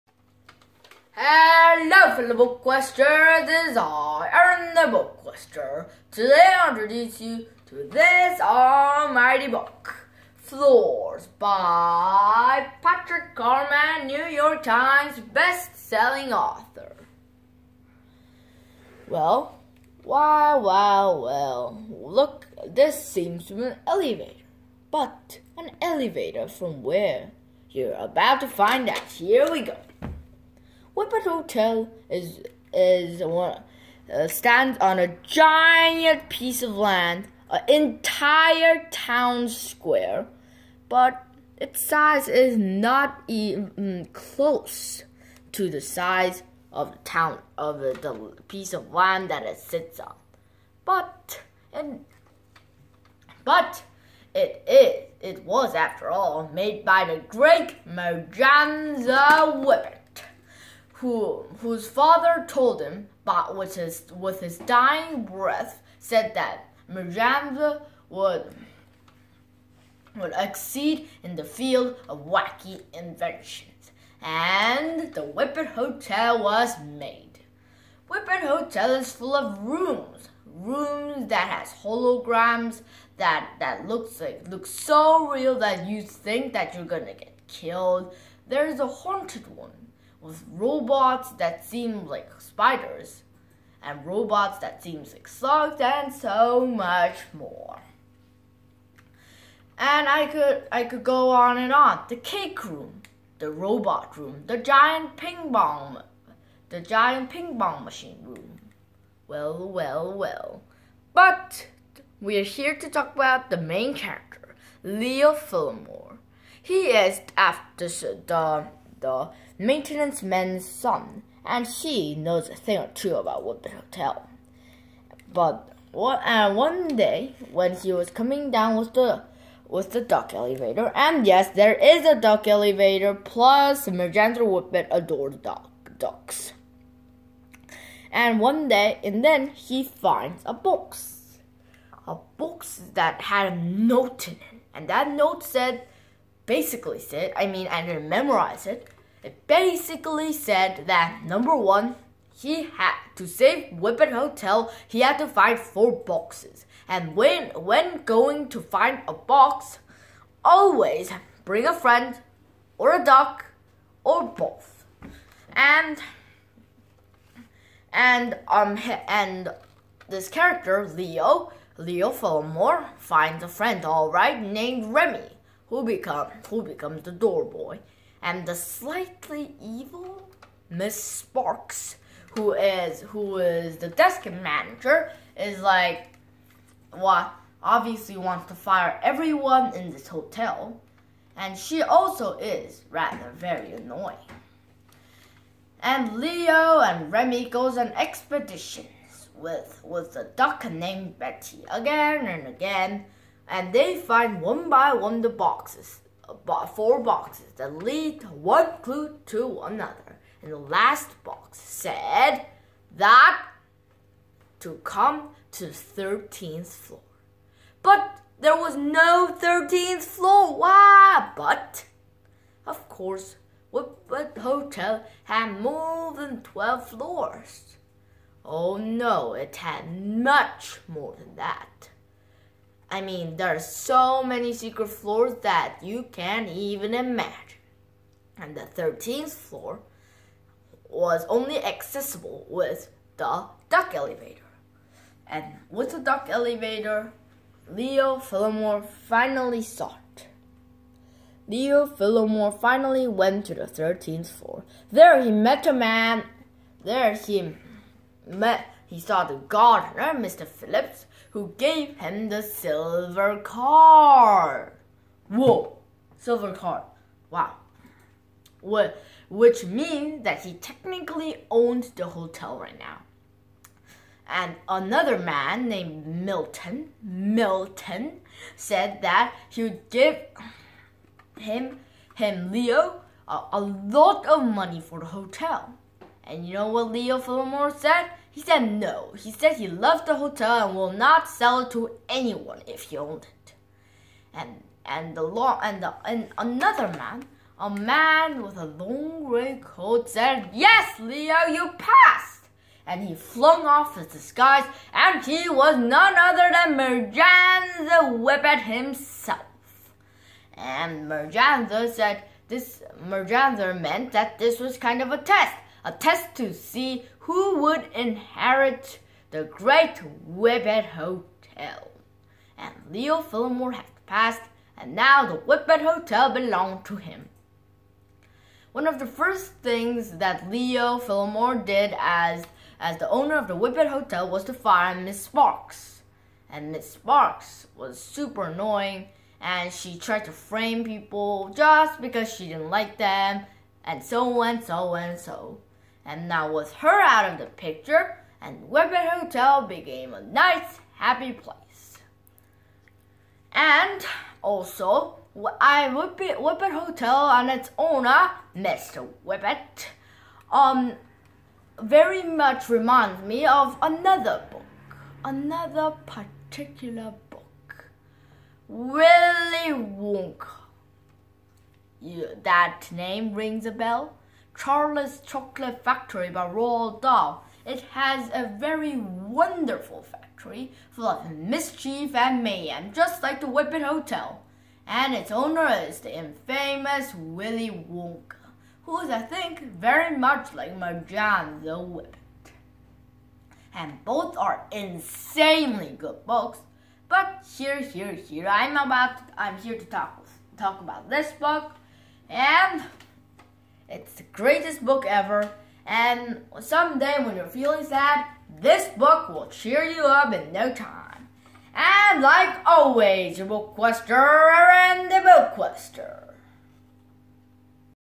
[BookQuester] 'Floors'...Something very mysterious is happing at the Whippet Hotel...
- by Patrick Carman ( 2011, 261 p )
- Mystery, Fantasy
- [영어책읽어주는 형아, 북퀘스터]